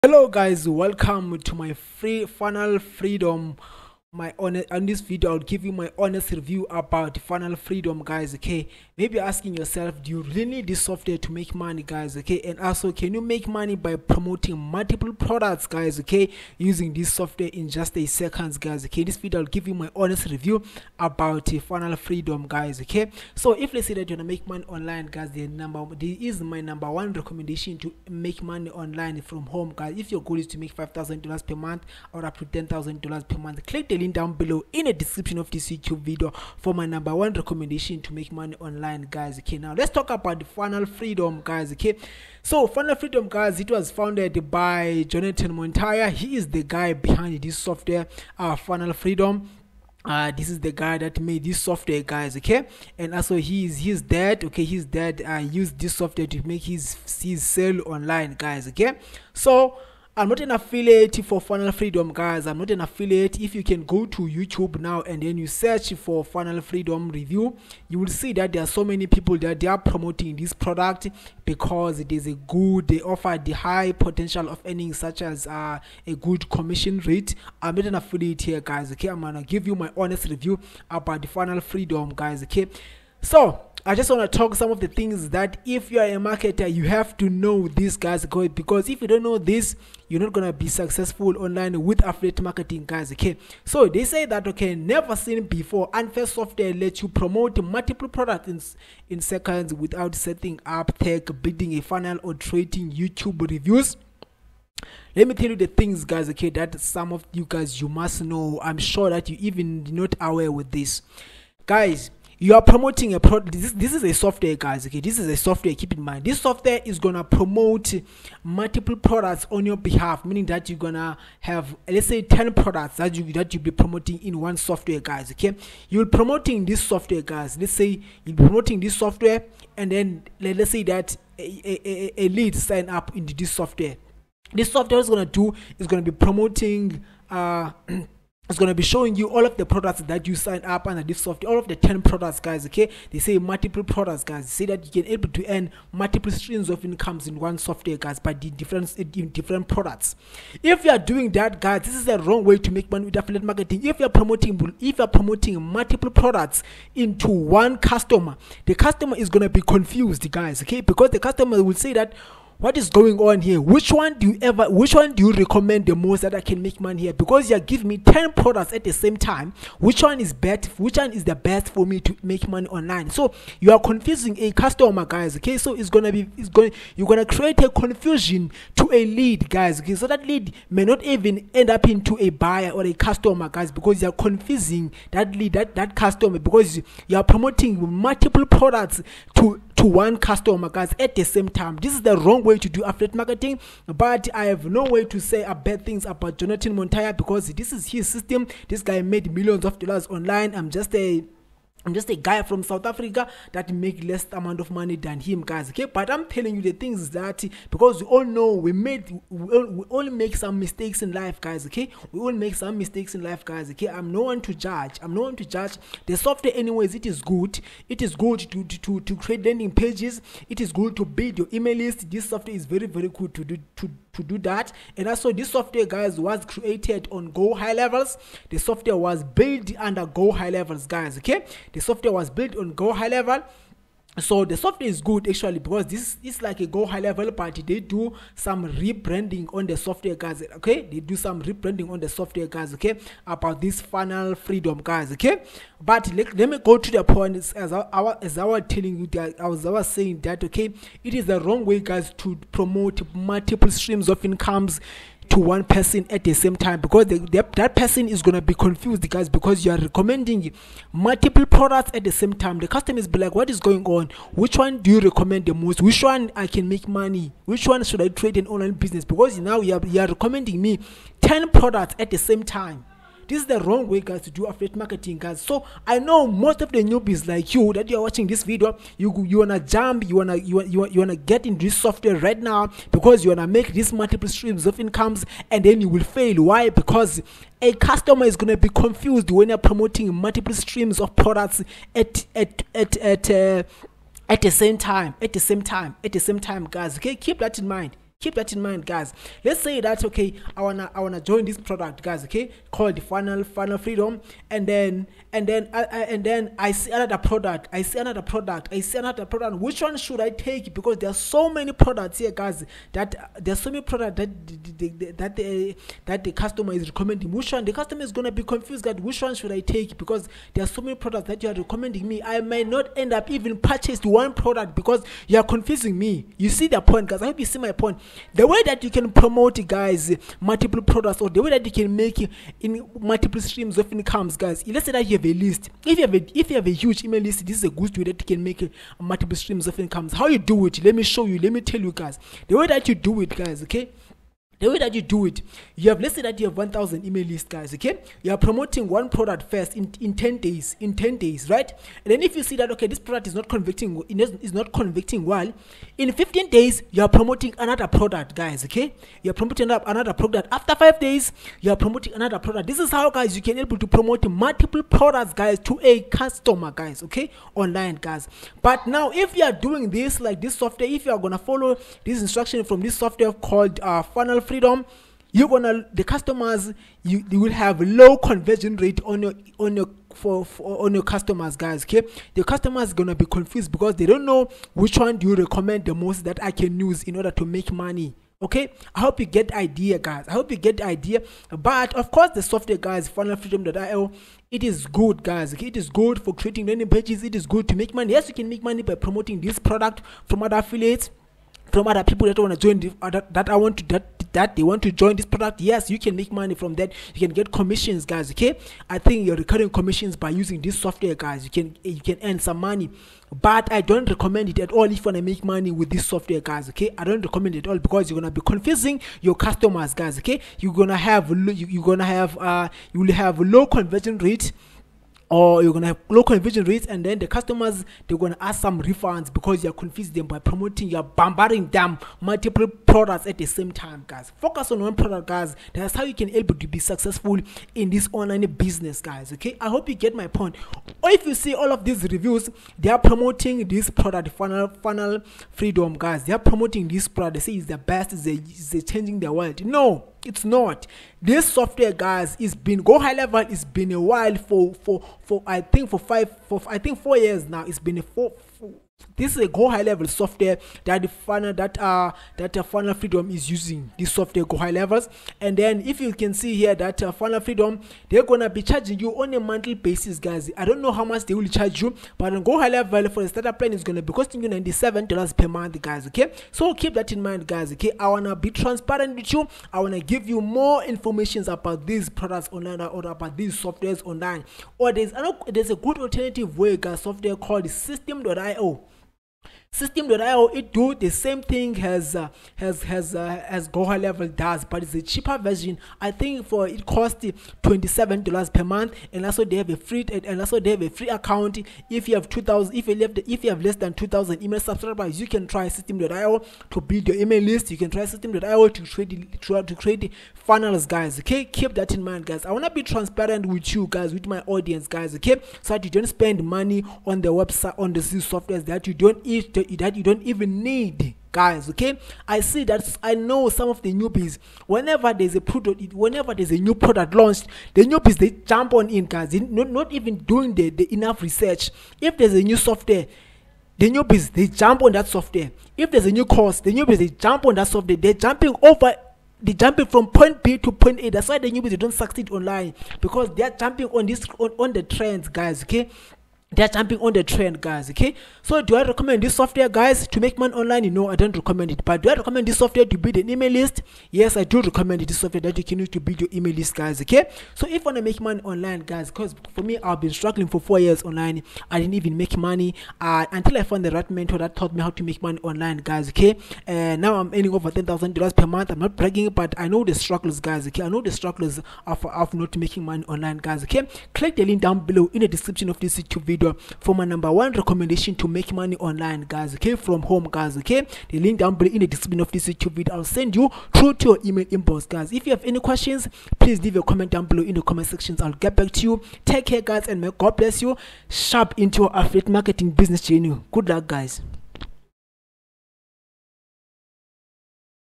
Funnel Freedom.io Review | Is Funnel Freedom Legit Or Scam
In this video I will talk about Funnel Freedom.io Review, you can promote more products using Funnel Freedom to earn money online for your business. But I don't think you need this software to earn money online because it will confuse customers when you recommend more products at the same time.